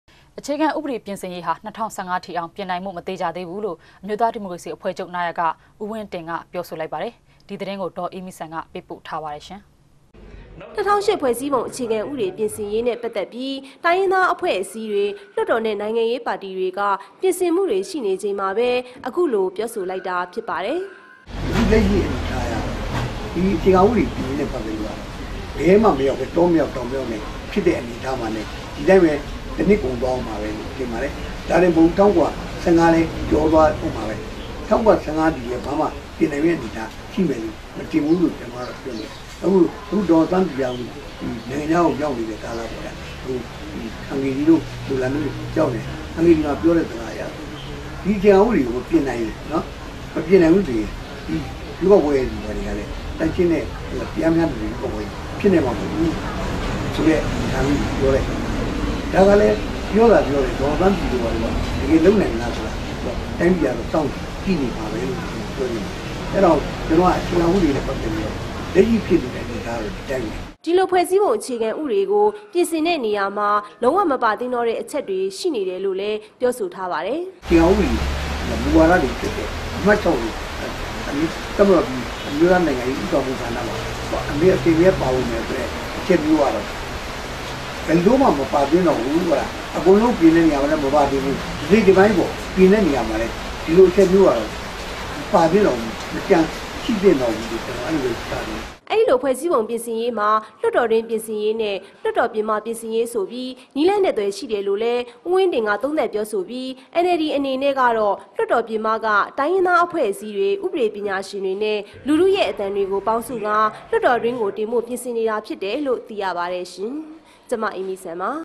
DVB - အေျခခံဥပေဒကို ၂၀၁၅အထိ ျပင္ႏိုုင္ဖိုု ့ မေသခ်ာ
DVB TV - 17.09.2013 - အေျခခံဥပေဒျပင္ဆင္ေရးဟာ ၂၀၁၅ ထိေအာင္ ျပင္ႏုုိင္ဖုုိ႔ မေသခ်ာေသးဘူးလို႕ အမ်ိဳးသားဒီမိုကေရစီအဖြဲ႕ခ်ဳပ္ရဲ႕ နာယက ဦး၀င္းတင္က ေျပာဆိုလိုက္ပါတယ္။ ဒီသတင္းကို ေဒၚေအးမိစံက ေပးပို႕ထားပါတယ္။